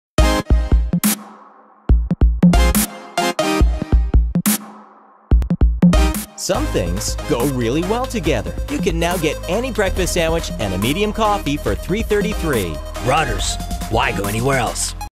Rutter's Farm Stores Breakfast Coffee Combo $3.33
Rise and shine at Rutter’s! For a limited time, use your Rutter’s Rewards card or VIP information and get any breakfast sandwich and a medium coffee for just 3.33! Start with high-quality meats and cheeses on a fresh baked roll then add your choice of Rutter’s delicious toppings. Pair it with a medium cup of Rutter’s gourmet coffee! Brewed fresh all day, every day you can make each cup just the way you like it using Rutter’s one of a kind coffee bar. Earn 2 cents per gallon for the combo! We’ve got breakfast covered!  Rutter’s, why go anywhere else?